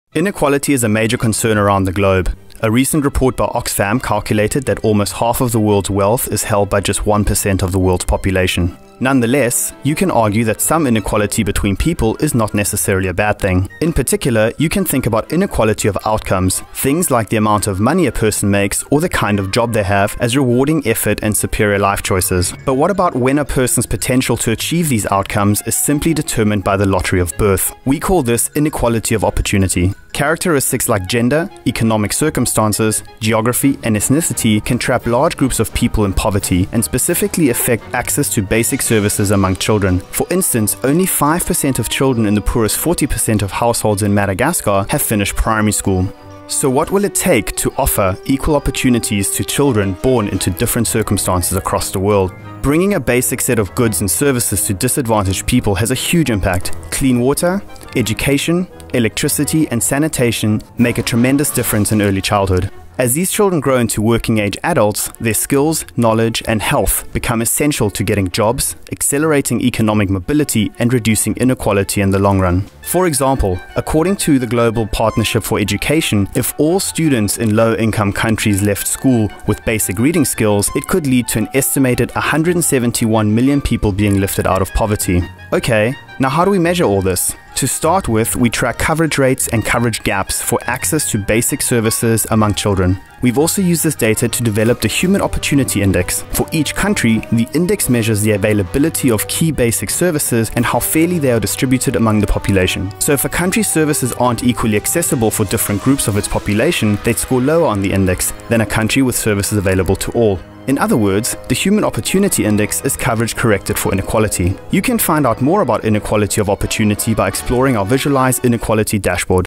The Data Minute: What is Inequality of Opportunity?
- What happens when a person’s potential to succeed in life is simply determined by the lottery of birth? We call this Inequality of Opportunity. Characteristics like gender, economic circumstances, geography, and ethnicity can trap large groups of people in poverty, and specifically affect access to basic services among children. Watch this video to learn more about Inequality of Opportunity--what it is, how we measure it, and where you can find more information.